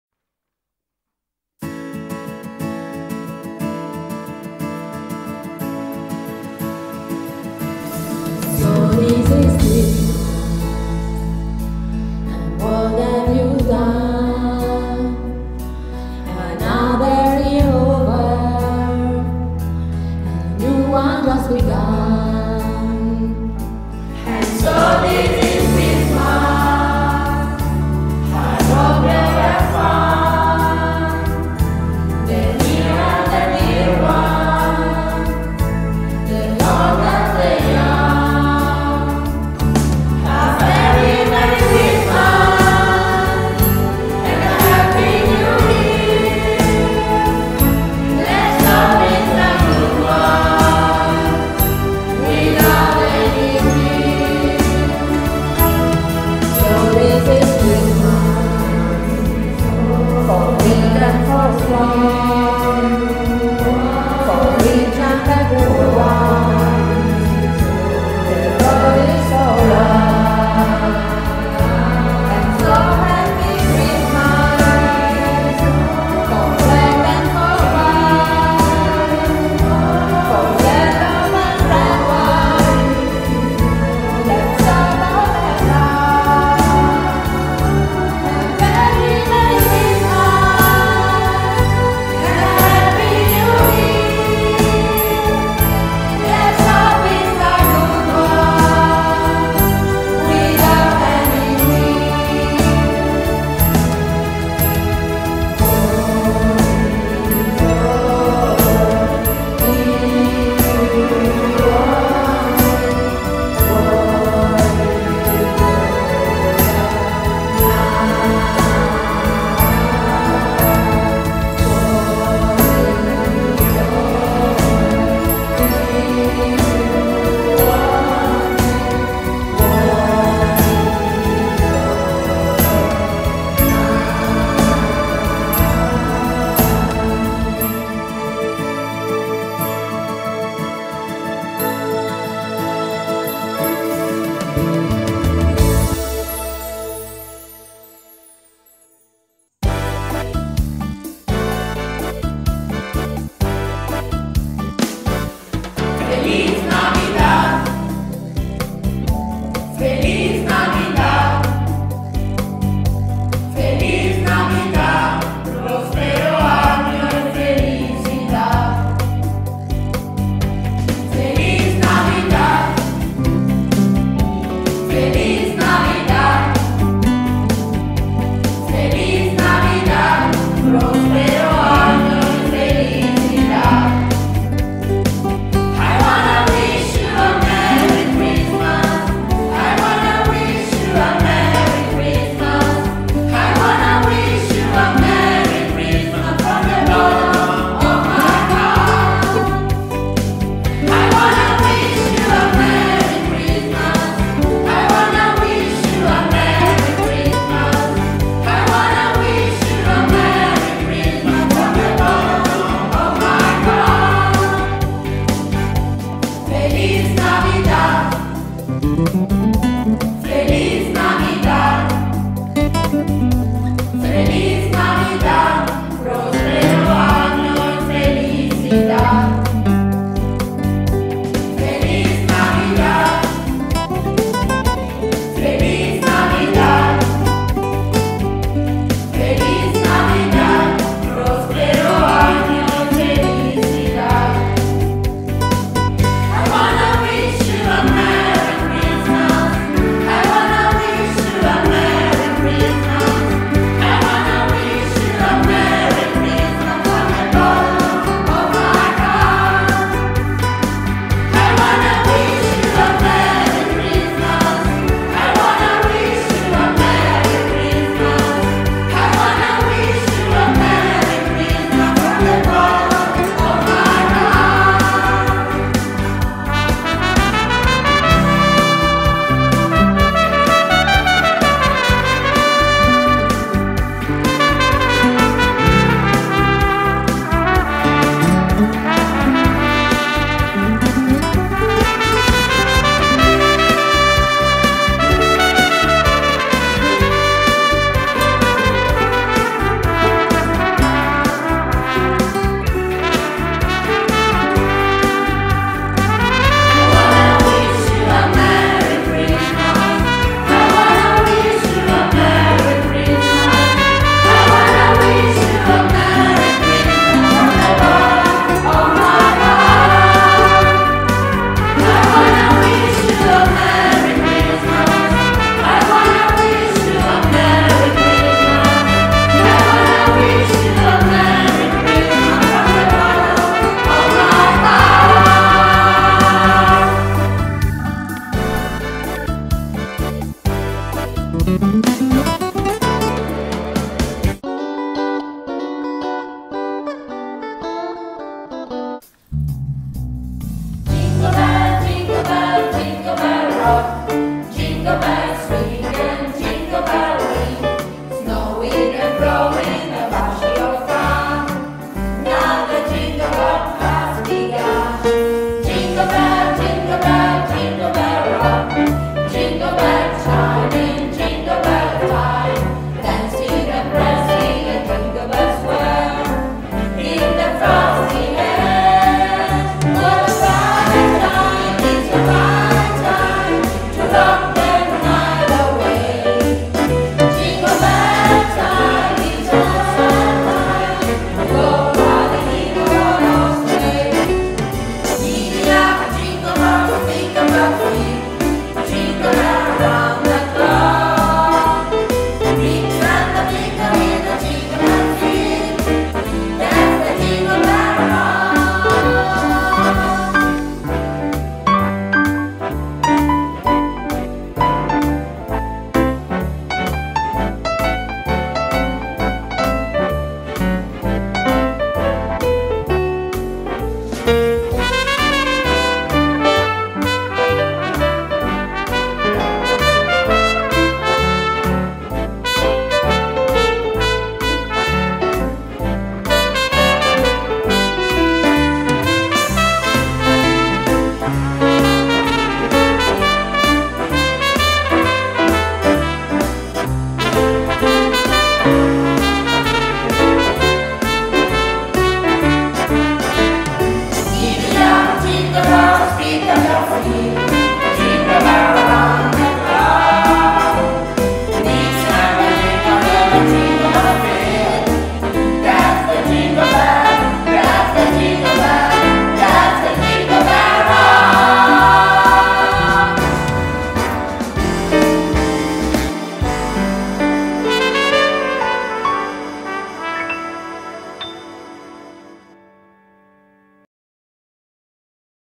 Auguri di Buone Feste dall'IISS Piera Cillario Ferrero di ALBA - A.S. 2021/22
Realizzato dalla classe II D con i proff. Antonella Bertolino, Mario Zappalà (arrangiamenti, tromba e montaggio video), con la partecipazione straordinaria dei proff. Vincenzo Corino (pianoforte), Anna Bocchino e Alessandro Martorana (voci). Ringraziamenti particolari alla Dirigente Paola Boggetto.